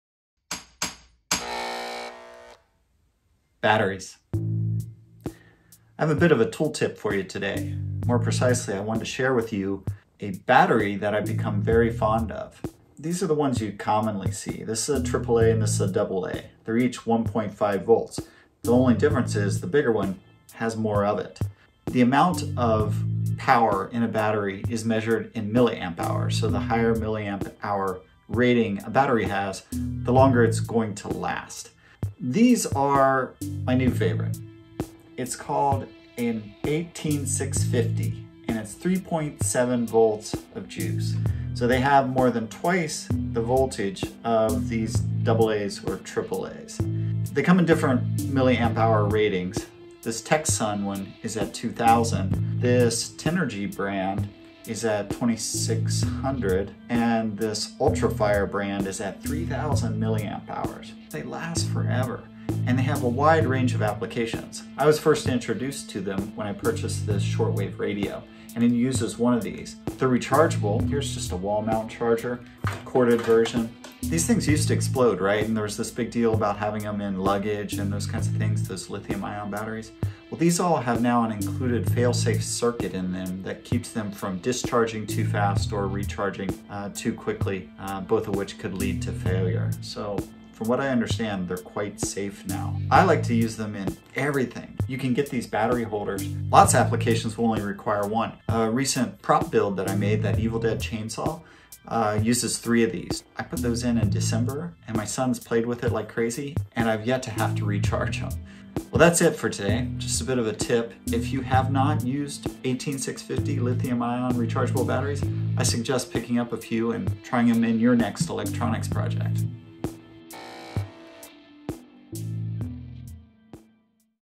Use a better battery! Today on What's he building in there? 2021.03.18
Ever heard of an #18650 Lithium-ion battery? These less common power cells are loaded with juice and perfect for many projects. If you make anything with electronics, you should get to know these power-packed power sticks. Today on What's he building in there? The daily VLOG by maker and writer J Robert Clark.